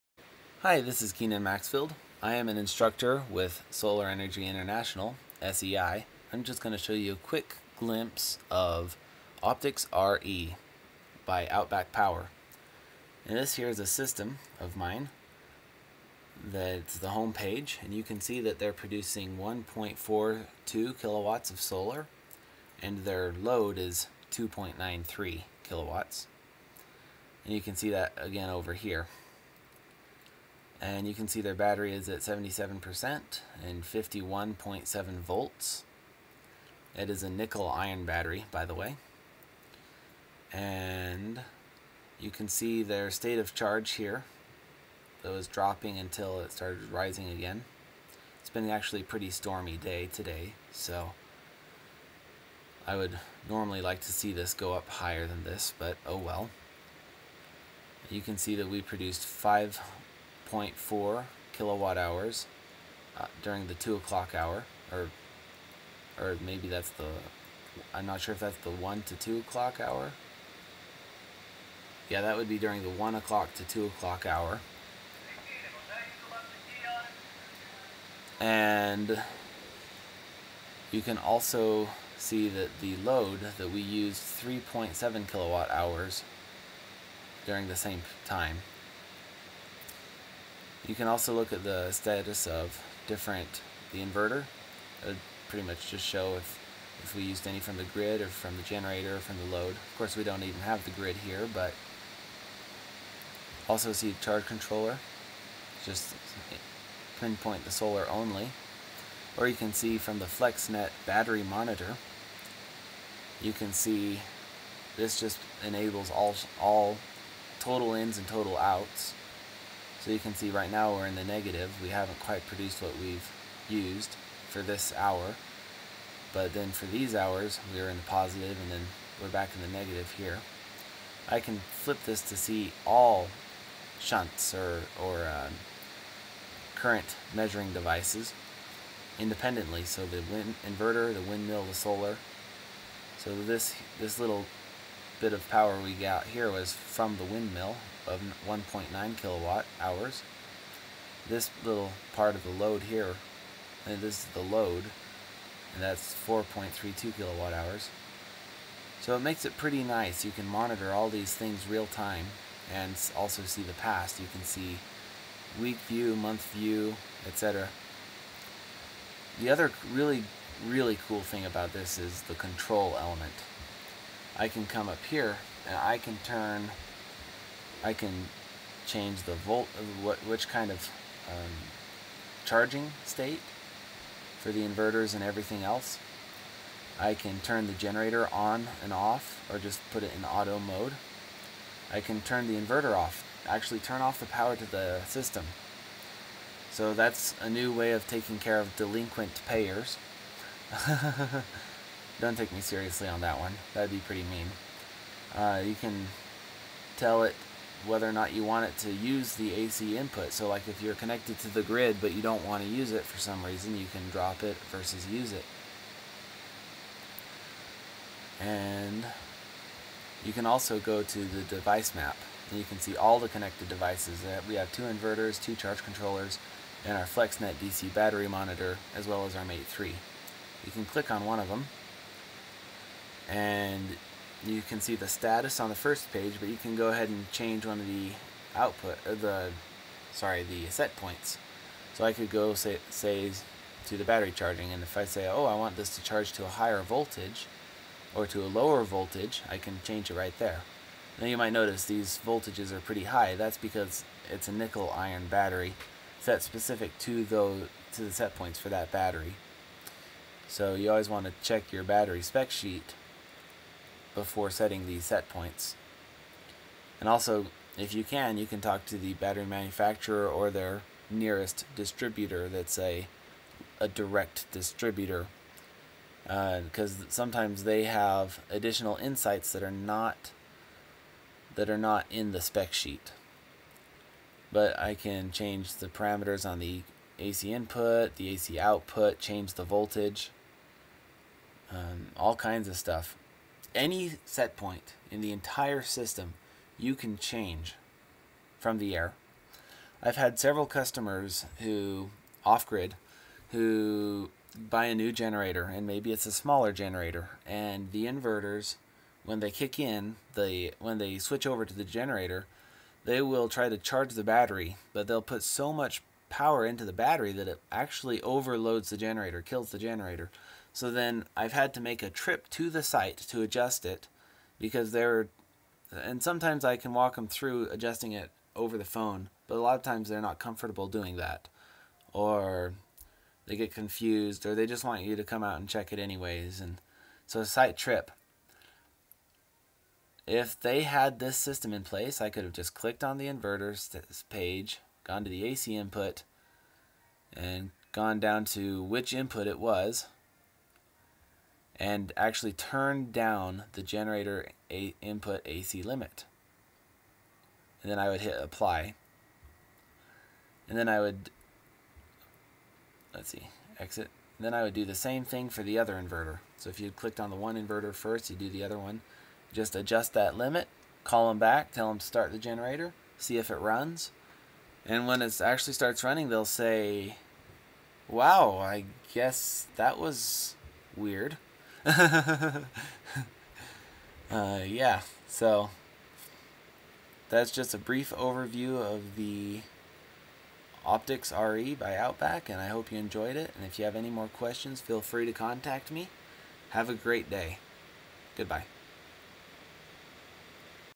Optics RE overview
This is a brief overview of Outback Power's Optics RE system monitoring and control.